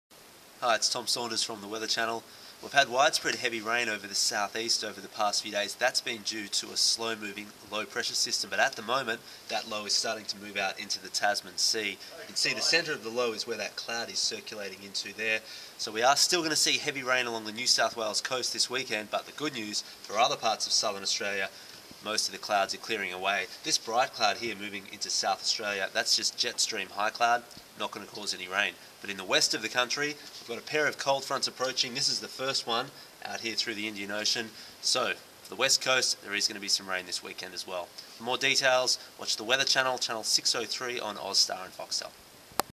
Australia Weather Update - 19 August 2011 AM - The Weather Channel
The slow moving low-pressure system that soaked south-eastern Australia is starting to move out into the Tasman Sea. This means more heavy rain along the coast of New South Wales this weekend. Meanwhile, a pair of approaching cold fronts means more rain for the west coast this weekend.